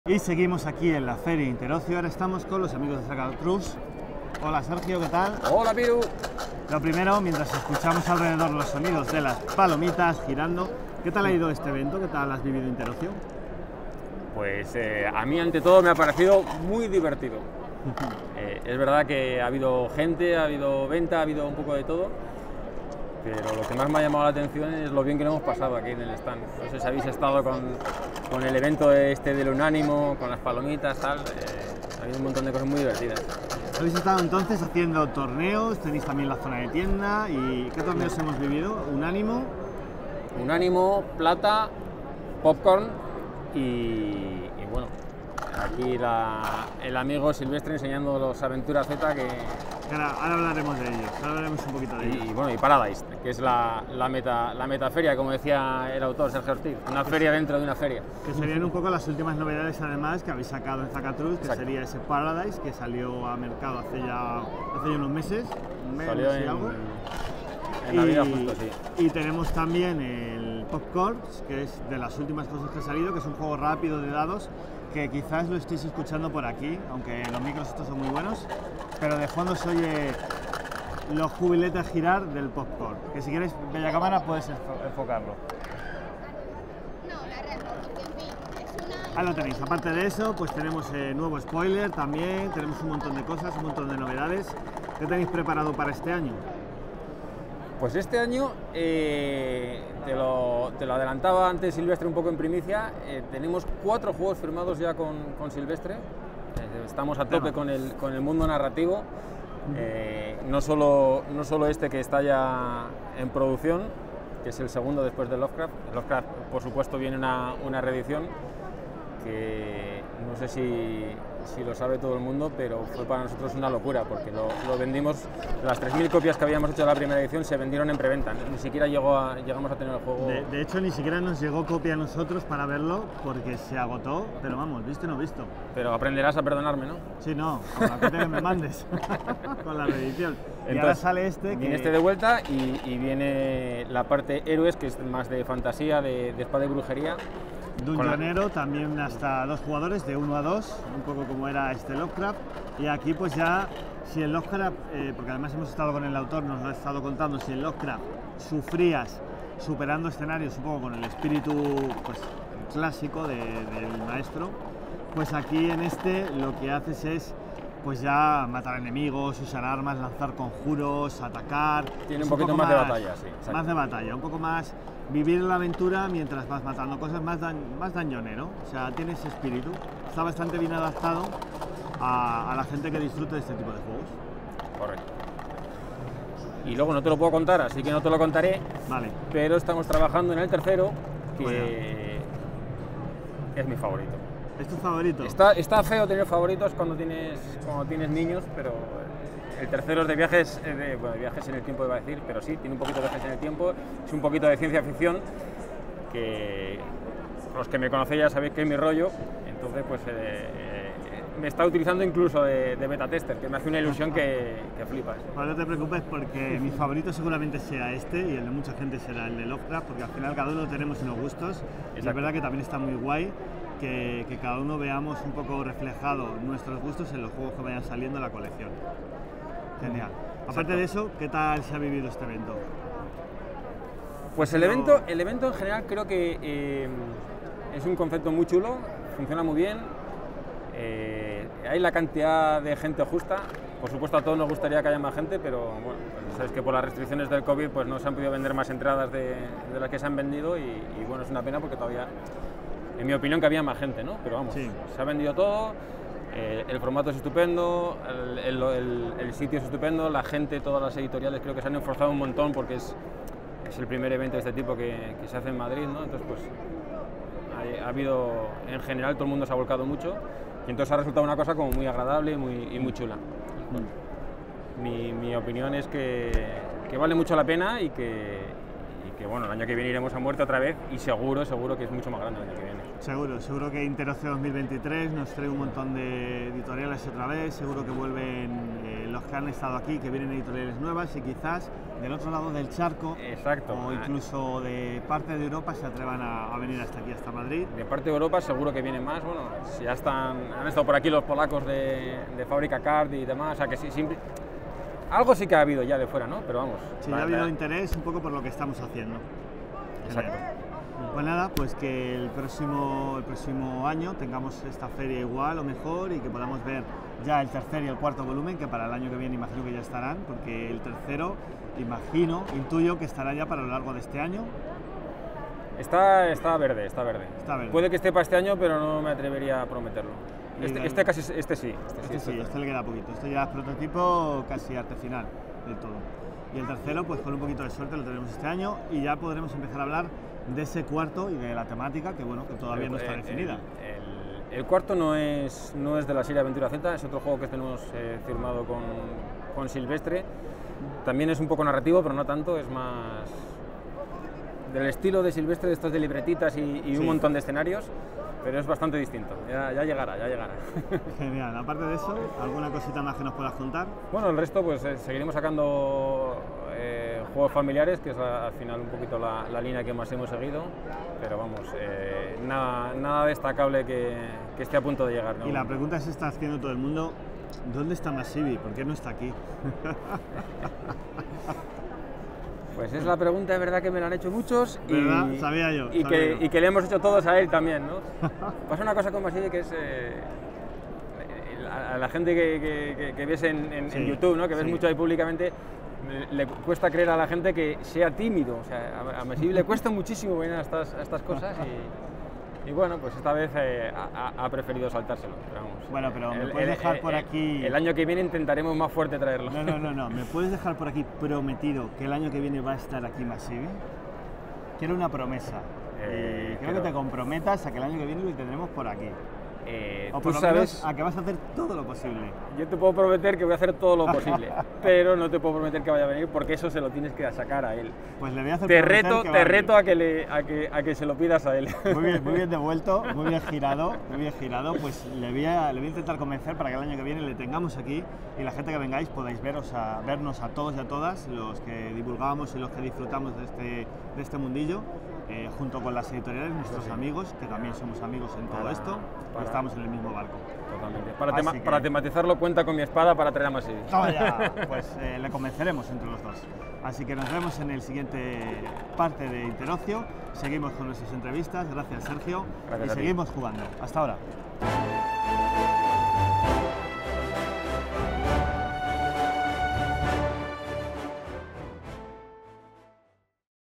¡Así son... al Natural! 👉 Entrevista a Zacatrus (Especial Interocio 2022)
¡Seguimos disfrutando de lo lindo en esta feria con gran compañía!

👇👇👇 Si te ha gustado el vídeo o nuestro trabajo... ¡Ayúdanos a seguir creciendo!